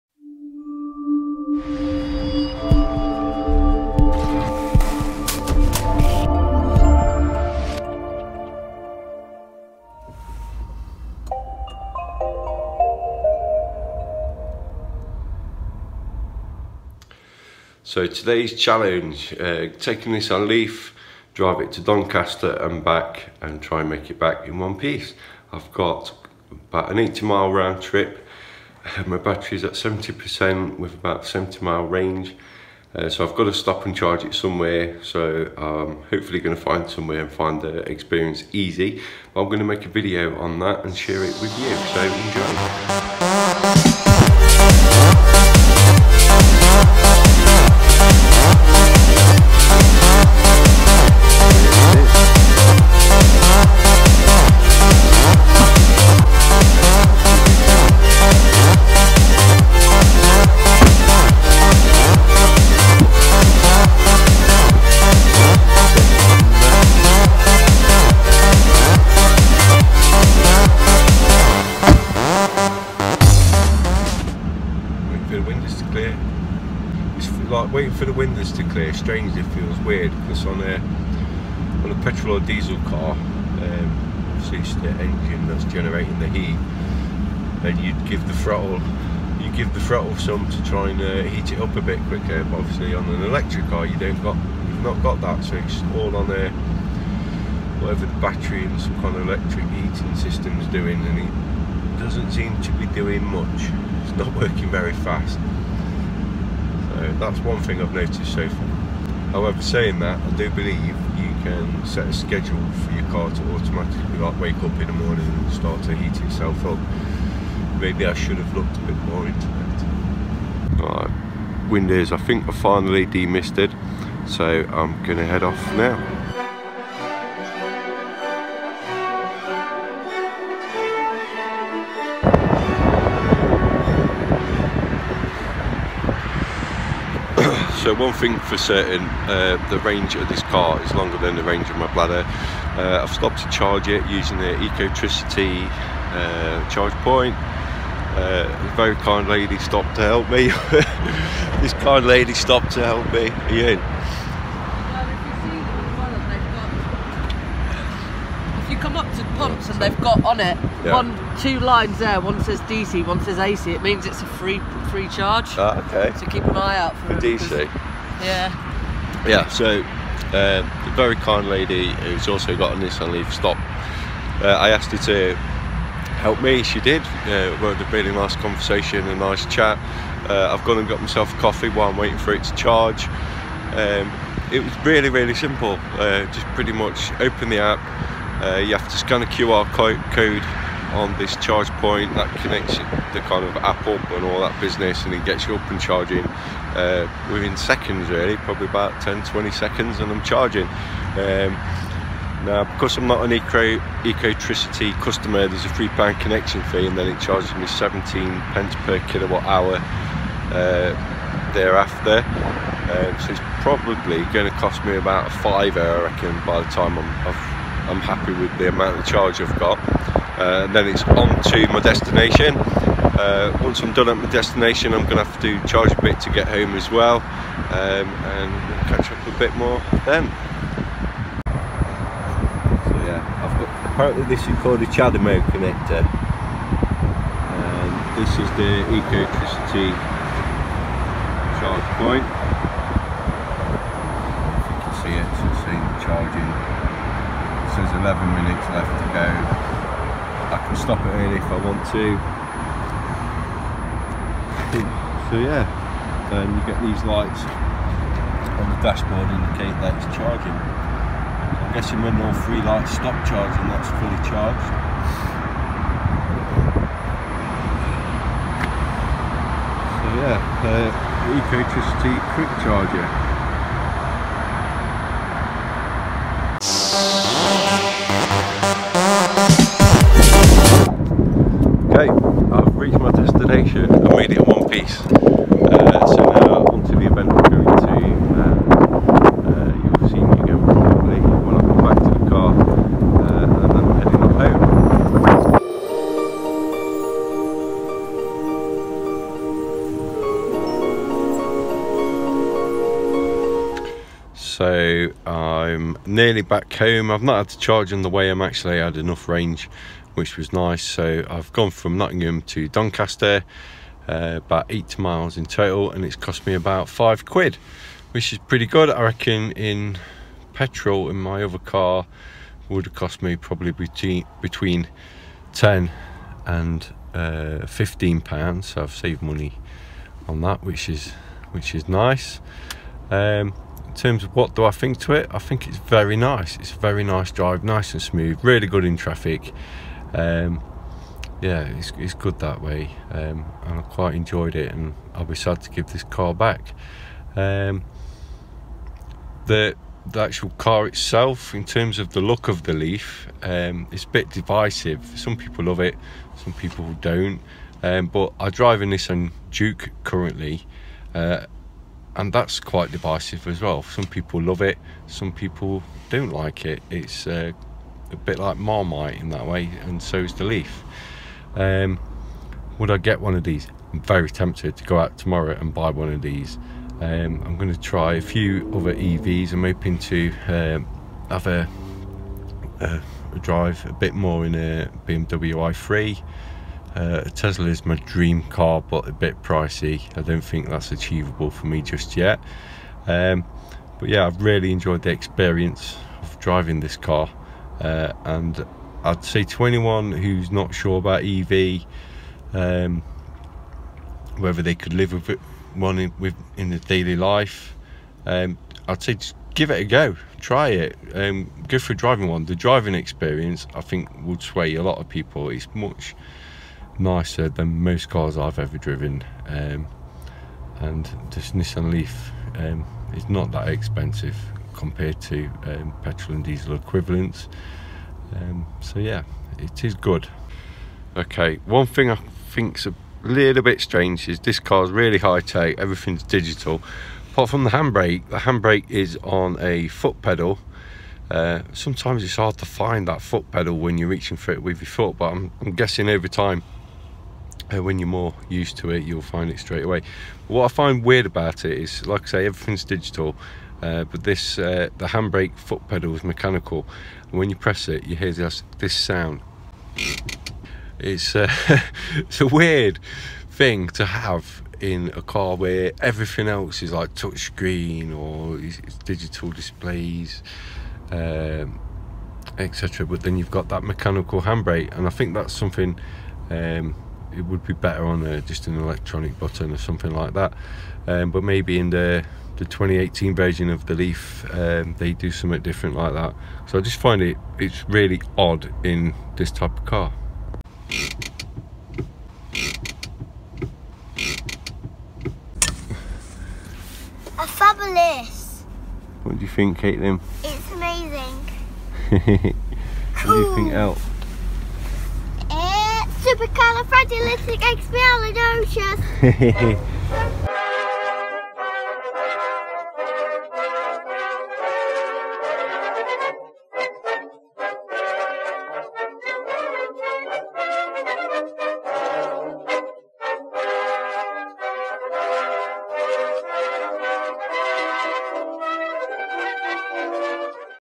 Nissan Leaf - A Smarter way of driving
Recently I got the chance to borrow a Nissan Leaf EV for the day. I felt that as someone with a smart home, I would also like a smart way of driving about, and I love the idea of owning an Electric vehicle so trying out the leaf was a great way to see how it would fit into my life and it the range would be enough for me. I drove 80 miles from Nottingham to Doncaster and back, and I flipping loved it.